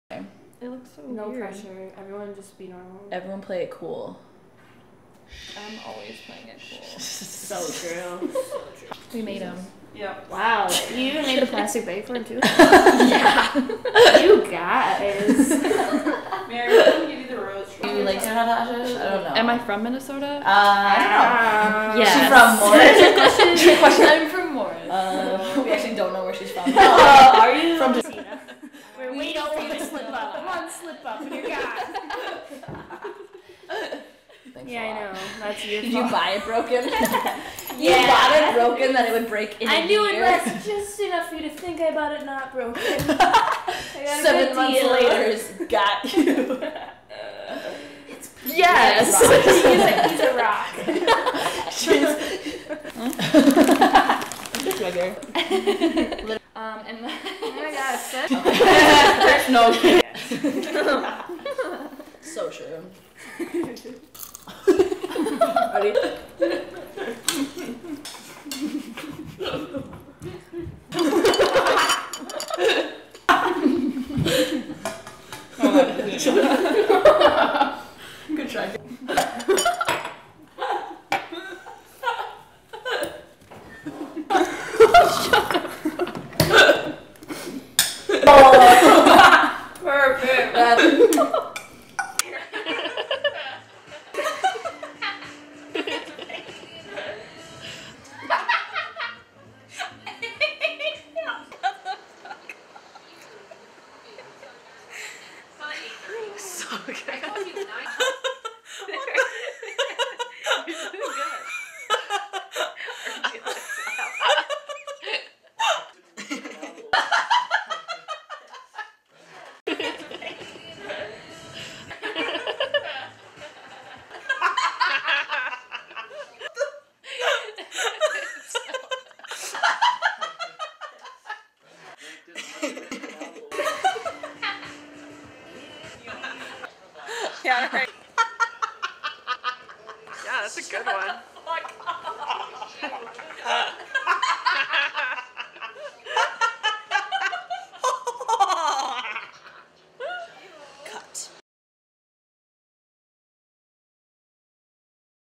Story of Self: Samantha
"Story of Self" is the first of three projects completed by students in Making Media Making Change, a joint course offered by SPNN and HECUA. It illustrates a changing point in the student’s life that helped define their values and catalyzed a commitment to action.